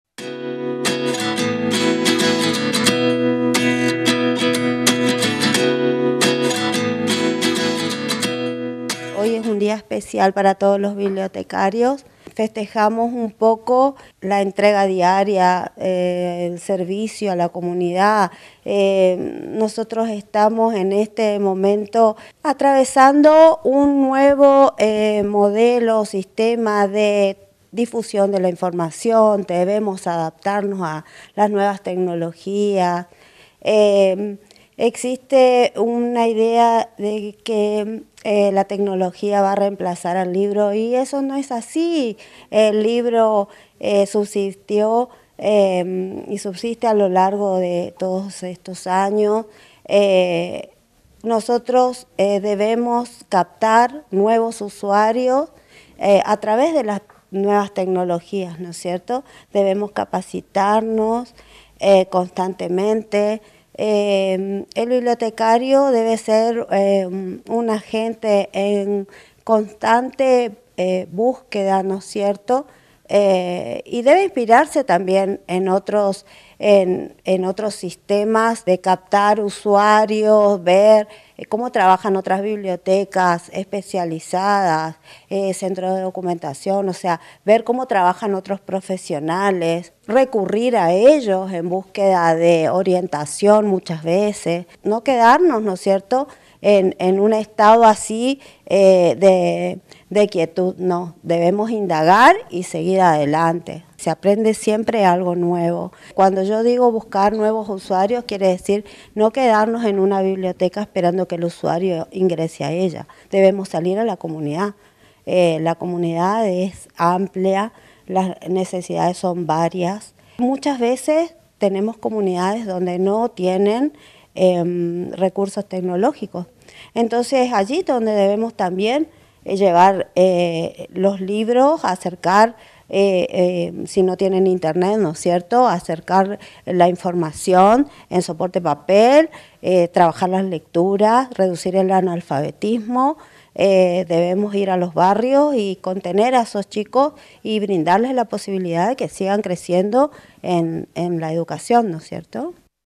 DÍA DEL BIBLIOTECARIO - NOTICIERO 9 -
¿CÓMO ES LEER EN ÉSTA ERA DIGITALIZADA?
Cada 13 de septiembre se celebra en nuestro país el Día del Bibliotecario ya que fue ese día en 1810 que la junta de gobierno a cargo de Mariano Moreno decidió crear la Biblioteca Pública de Buenos Aires. Dicho oficio, asegura Sandra, nos acerca al mundo de las letras, y hasta hoy en día, uno descubre algo nuevo jornada tras jornada. Pese a la nueva etapa tecnológica, los libros siguen subsistiendo, siendo una elección constante para los ciudadanos, y amantes de la literatura.
NOTA: SANDRA DÍAZ- DIRECTORA DE LA BIBLIOTECA MUNICIPAL